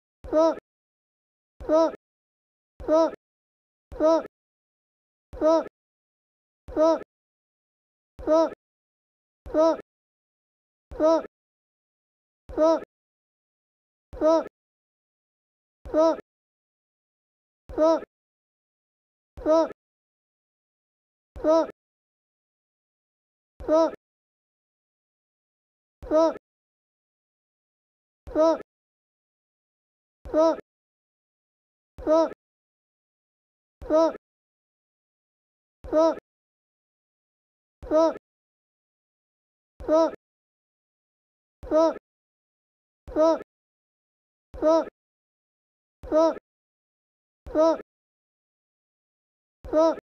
Do you hear cook or book?
Okay now do you hear cook or book? The answer is BABABABA! Okay that’s a joke it’s actually...

Book!